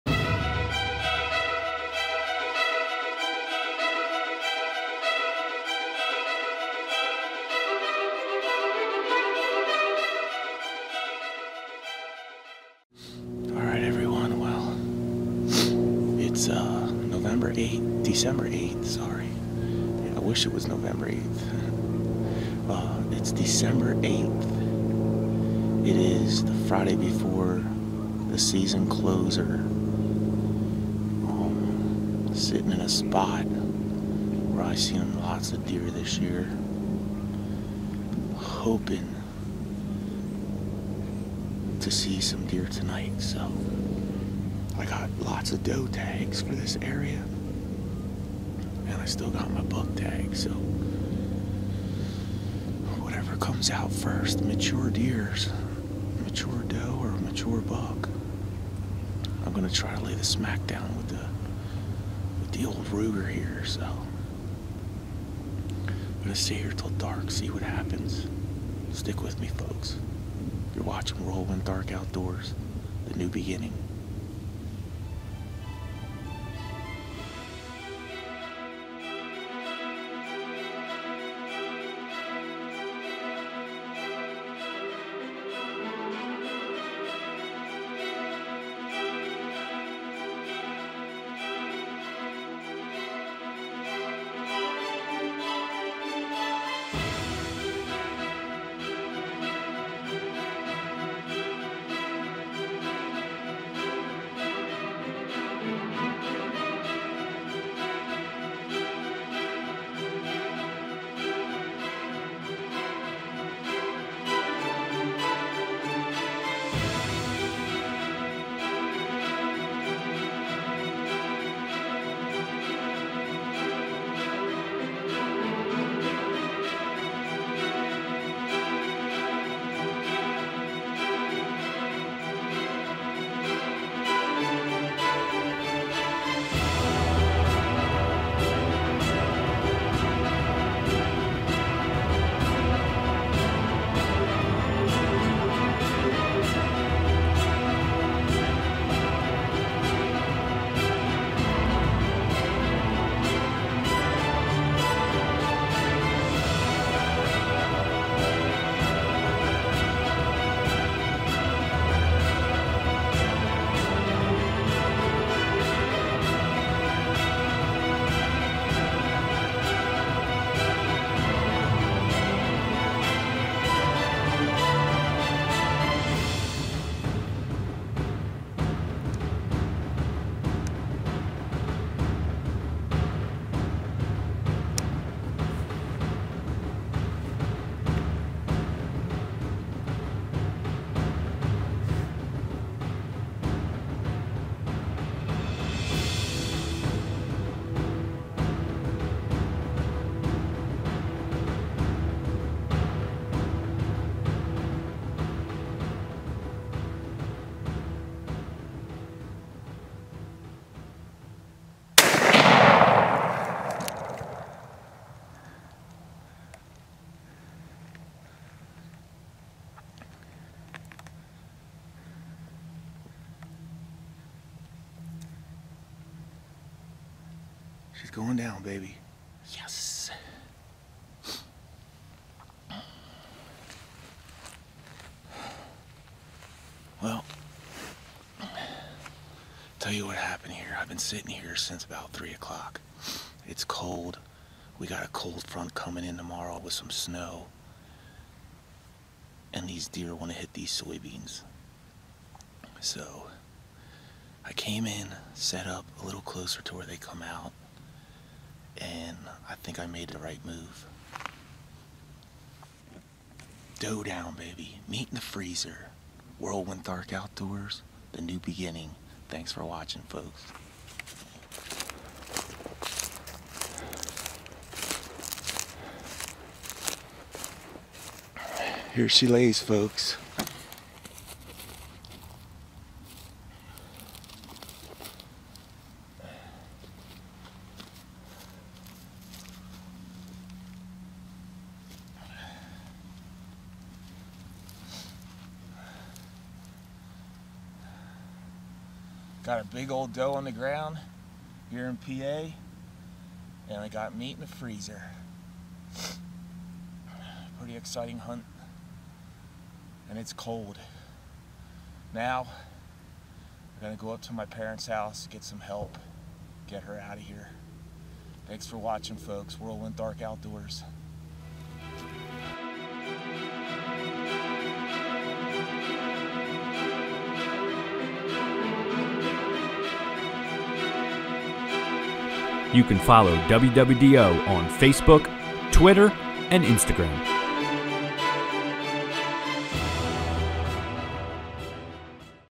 Deer Hunting PA. Episode 7 Season 4
Carlile Esterly AKA "The Slickhead Guru" lays the hammer down on a big old PA. Slickhead